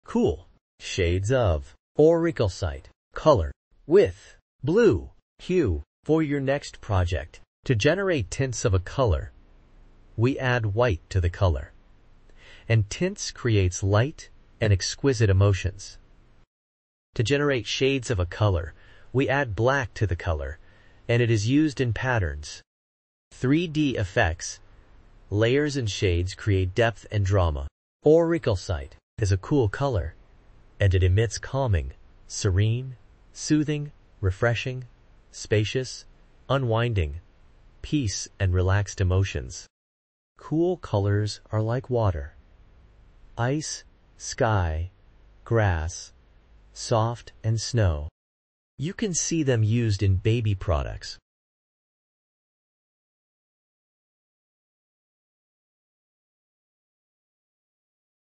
Shades & Tints of Aurichalcite color 32ffdc A Cool Blue color
Shades of Aurichalcite color 32ffdc Cool Shades of Aurichalcite color with Blue hue for your project!

Shades
Hex 2ce2c3

Tints
Hex 48ffdf

Aurichalcite is a cool color, and it emits calming, serene, soothing, refreshing, spacious, unwinding, peace and relaxed emotions. Cool colors are like water, ice, sky, grass, soft and snow. You can see them used in baby products.